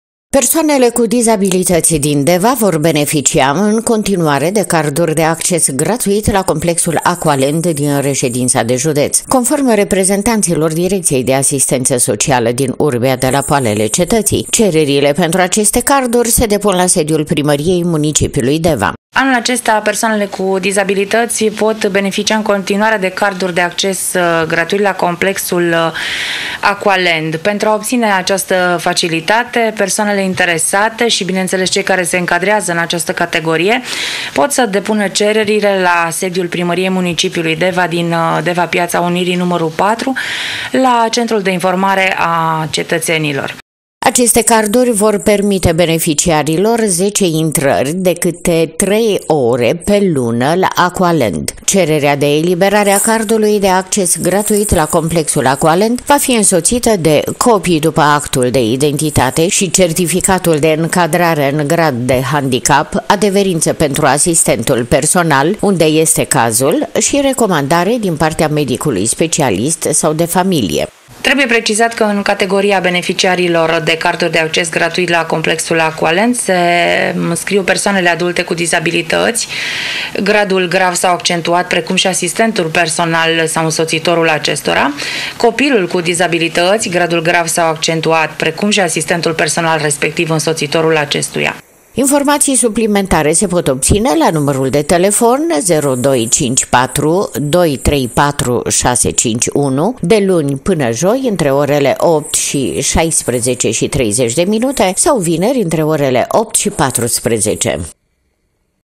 ACCES GRATUIT PENTRU PERSOANELE CU DIZABILITĂȚI LA “AQUALAND” DEVA
Persoanele cu dizabilități din Deva vor beneficia, în continuare, de carduri de acces gratuit la Complexul “Aqualand” din reședința de județ. Conform reprezentanților Direcției de Asistență Socială din urbea de la poalele Cetății, cererile pentru aceste carduri se depun la sediul Primăriei municipiului Deva.